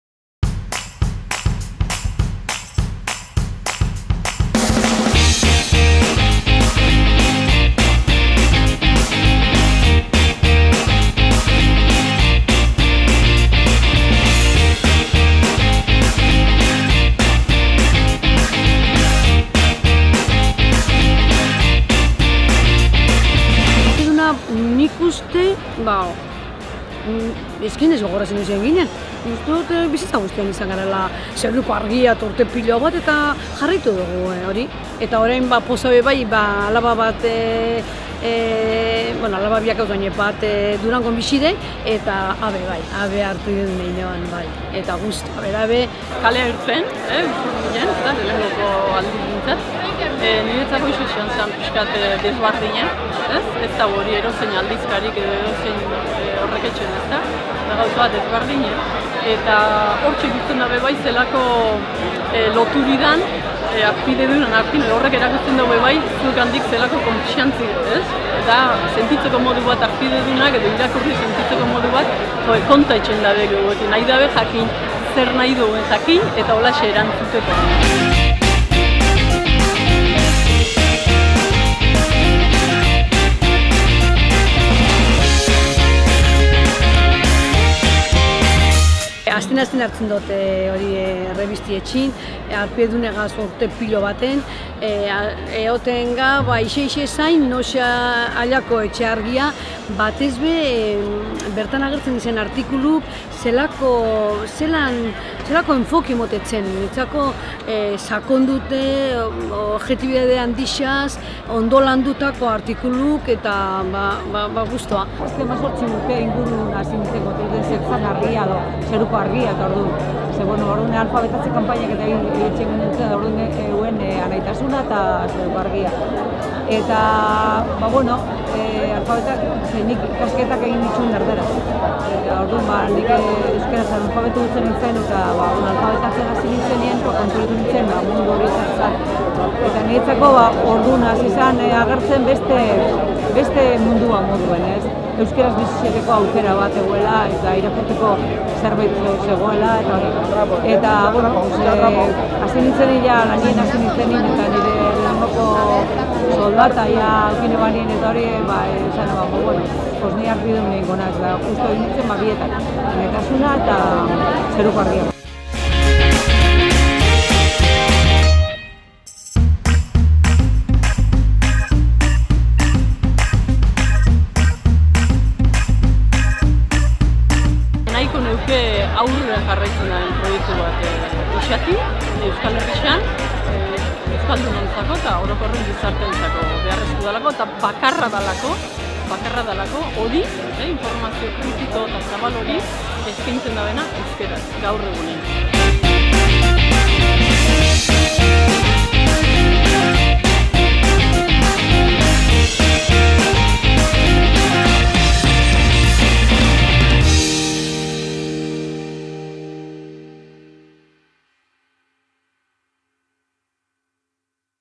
Argia Eguna Ondarroan 2016
Argia Eguna ospatu dugu Ondarroan. Balore jakin batzuk praktikan jartzen saiatu, komunitatea bildu, sostenguagatik eskerrak eman eta aurrera begira egin asmo ditugun proiektuez aritu gara egun polit honetan.